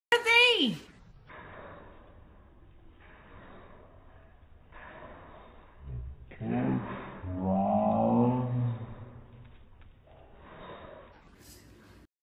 Dog slow mo prancing!
Zeus is about 4 years old and this how he walks when you give him a call!!!